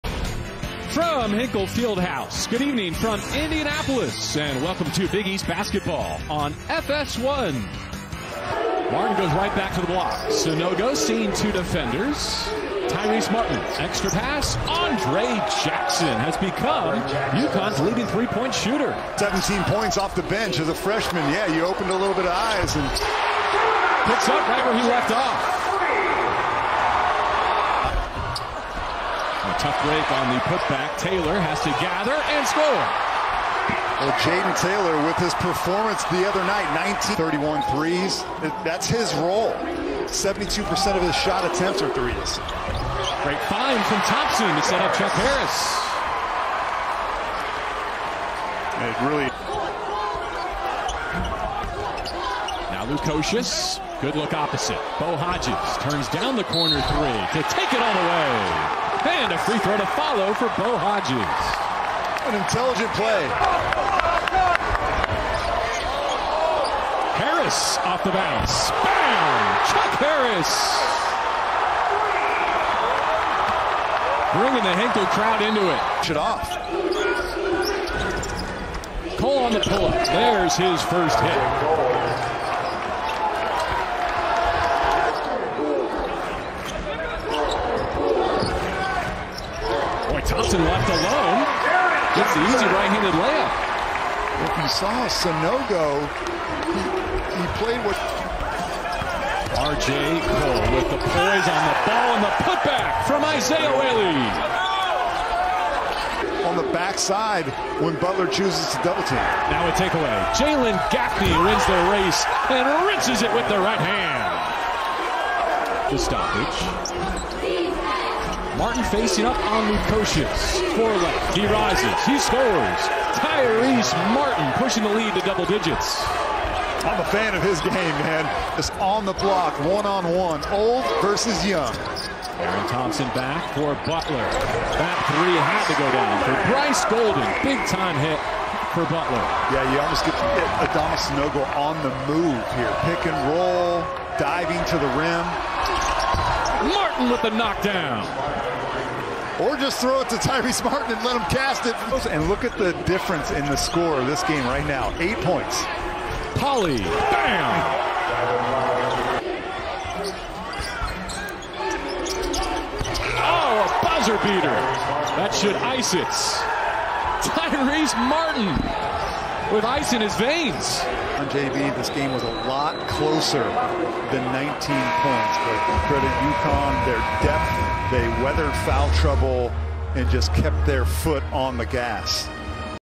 BIGEASThoops Highlights - #25 UConn 75, Butler 56
Tyrese Martin scored a career-high 27 points to lead UConn back from a four-point halftime deficit on Thursday night at Hinkle. The Huskies shot a blistering 66.7 percent from the floor in the second half, out-scoring the Bulldogs 51-28 over the final 20. The Dawgs got a team-high 16 points from Chuck Harris in the setback.